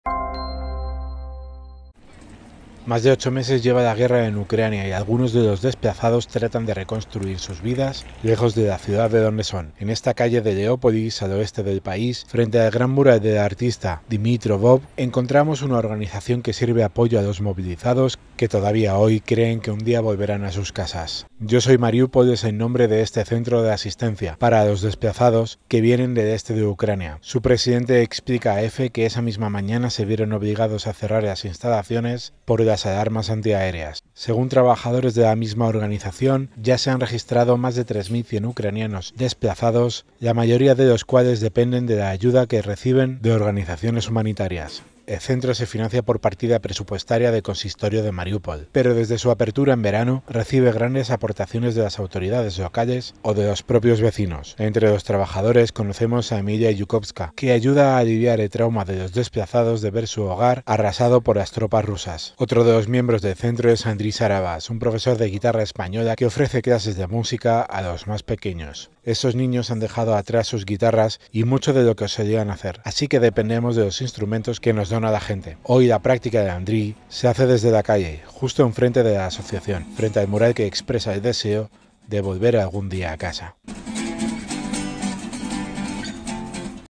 Los desplazados de Mariúpol encuentran refugio y apoyo en Leópolis
Los desplazados internos ucranianos de Mariúpol, enfrentados al trauma psicológico de tener que reconstruir sus vidas lejos de su ciudad semidestruida, dependen de su propia organización y del apoyo mutuo y creen que un día lograrán volver a casa.

¡Visítenos!